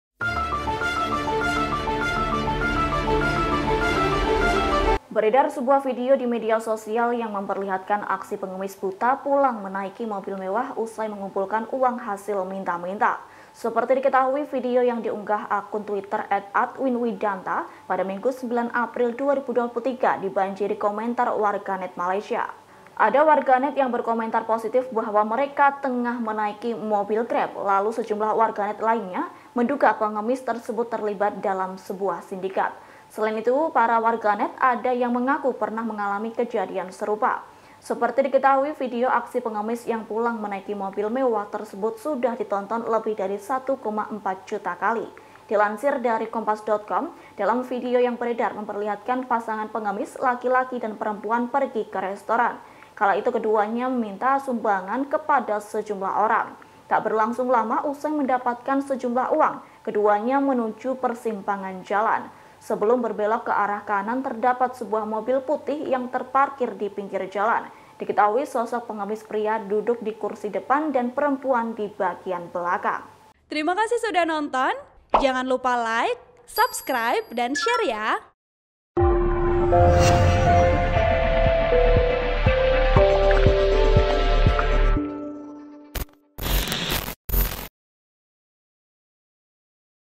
Tertangkap Kamera Aksi Pengemis Buta Pulang Naik Mobil Mewah seusai Kumpulkan Uang Hasil Minta-minta
Seperti diketahui, video yang diunggah akun Twitter @AdwinWidanta pada Minggu (9/4/2023) dibanjiri komentar warganet Malaysia.

Ada warganet yang berkomentar, bahwa mereka tengah menaiki mobil Grab.

“Dengan asumsi terbaik, mungkin mereka masuk ke mobil Grab,” tulis warganet Dharshan.

Lalu, sejumlah warganet lainnya menduga pengemis tersebut terlibat dalam sebuah sindikat.

(Tribun-Video.com/Kompas.com).

Vp: Indra